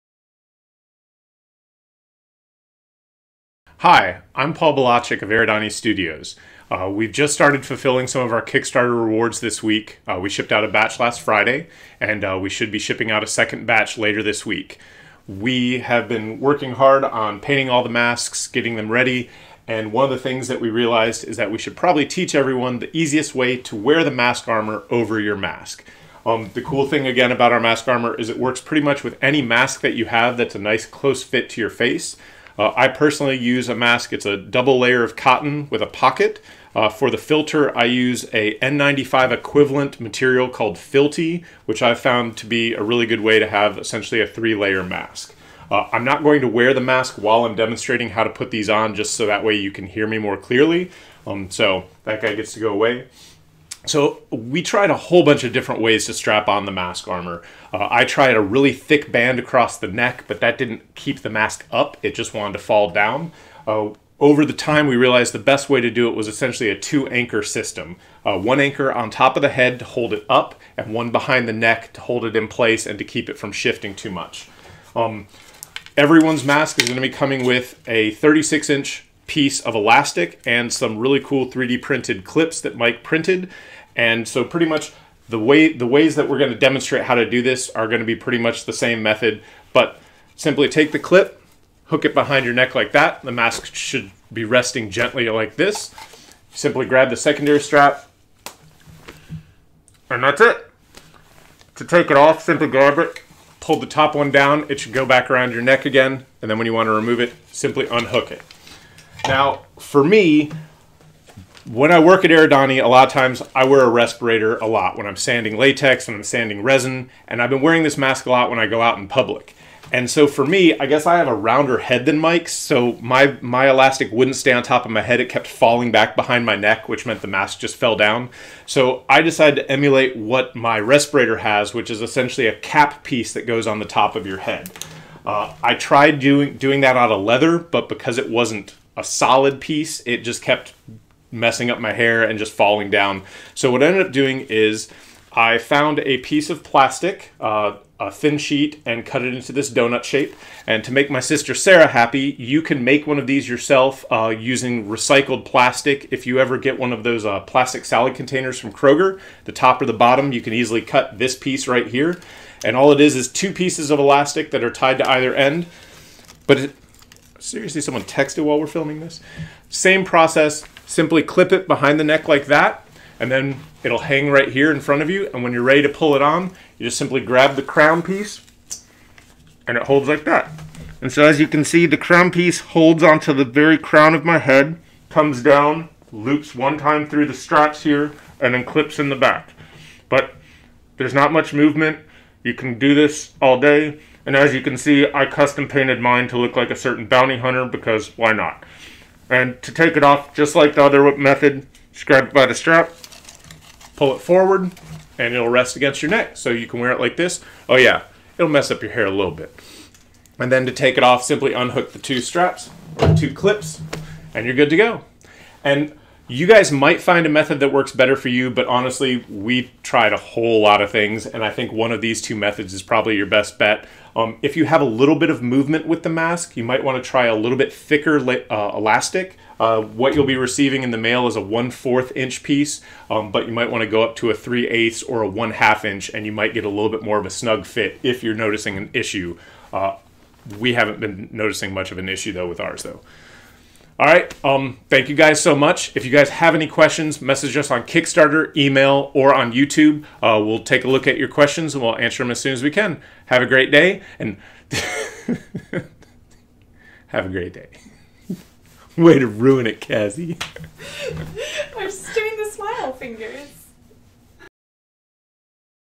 How to Wear Your Mask Armor
A short little tutorial that demonstrates how to properly put on your Aradani Studios Mask Armor.